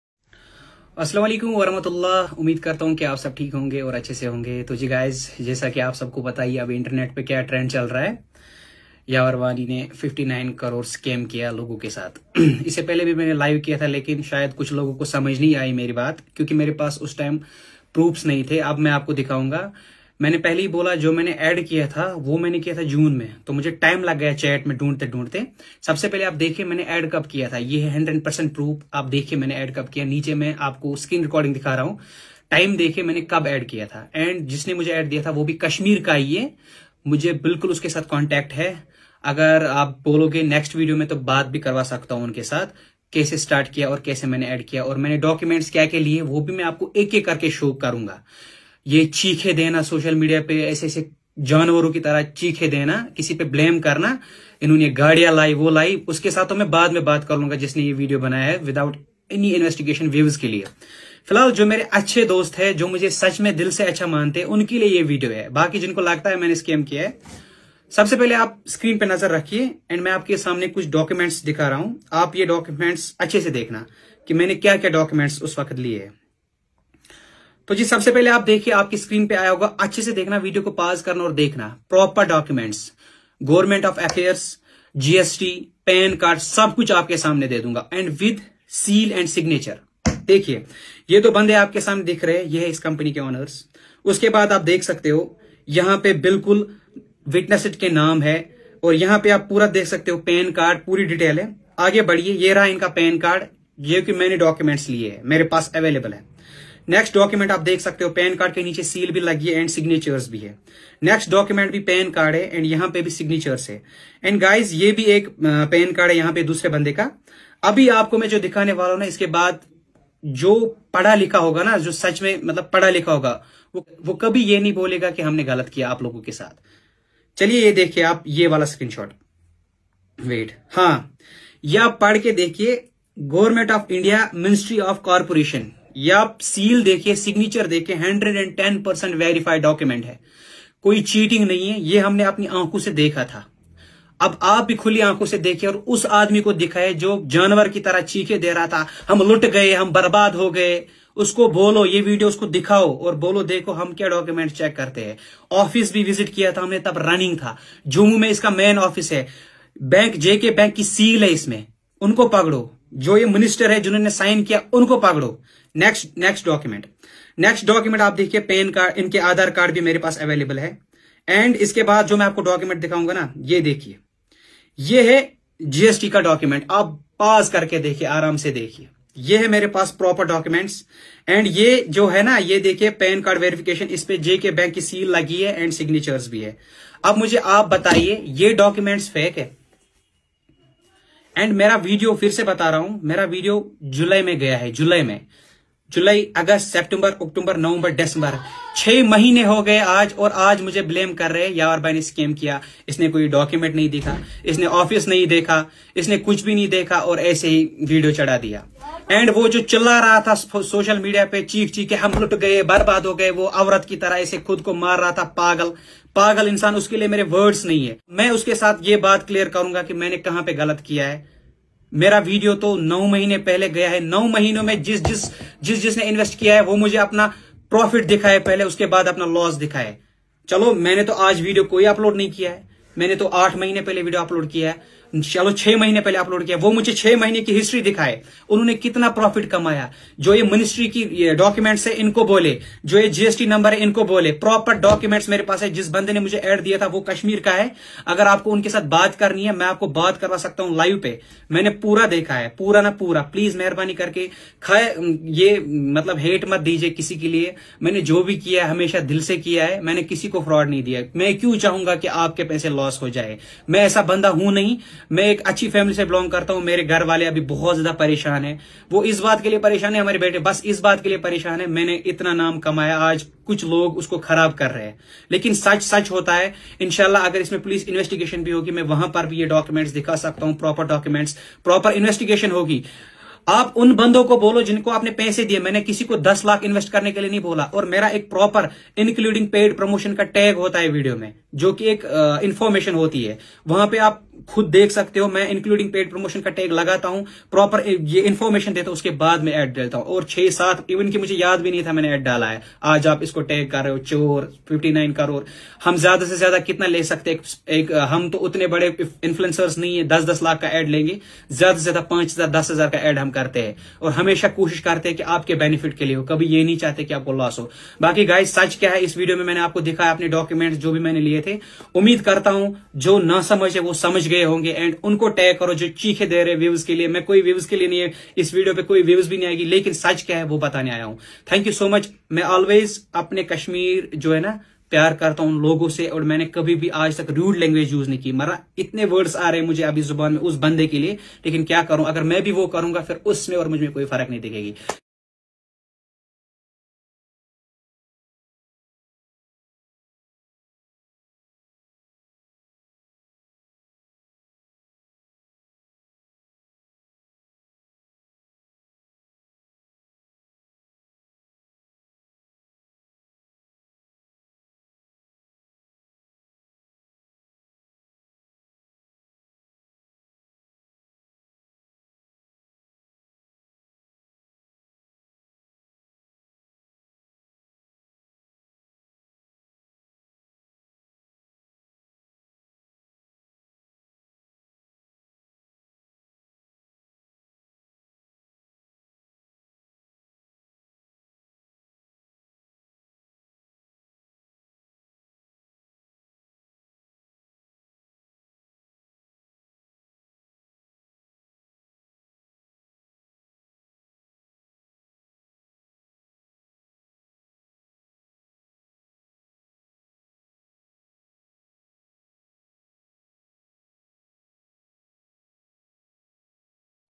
Yawer Wani's reply on CURATIVE SURVEY PVT LTD Scam / watch video
CURATIVE SURVEY PVT LTD FRAUD WEBSITE/59 crore scam with kashmiri people.